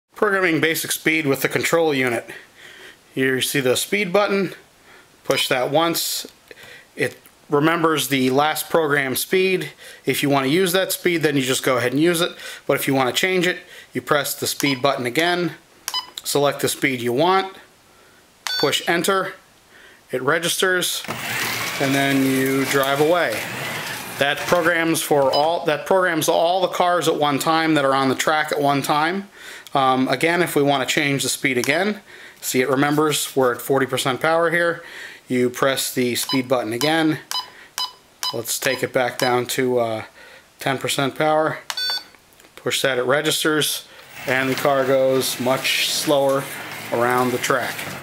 Carrera D132-Control Unit-Programming Speed
How to set the basic speed of each car using the new Carrera DIGITAL 132 Control Unit.